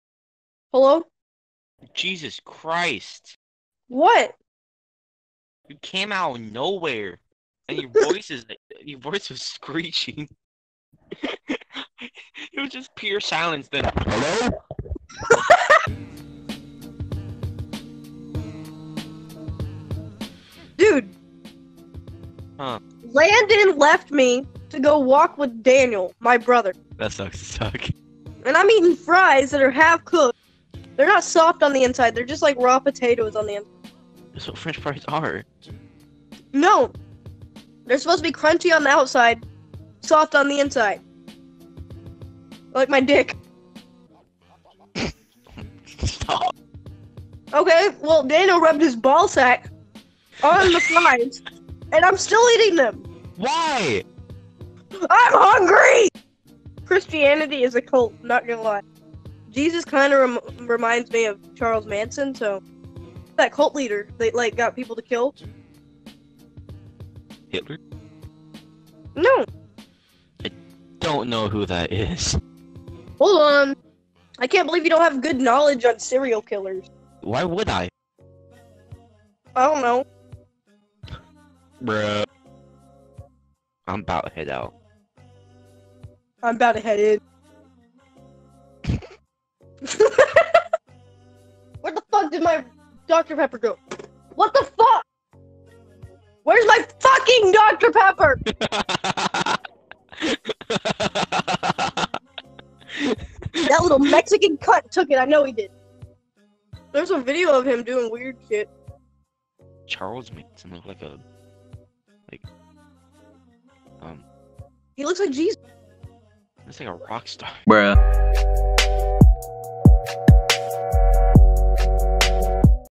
I recorded friends on discord and subtitled it.
Im gonna upload 2 mins of this every week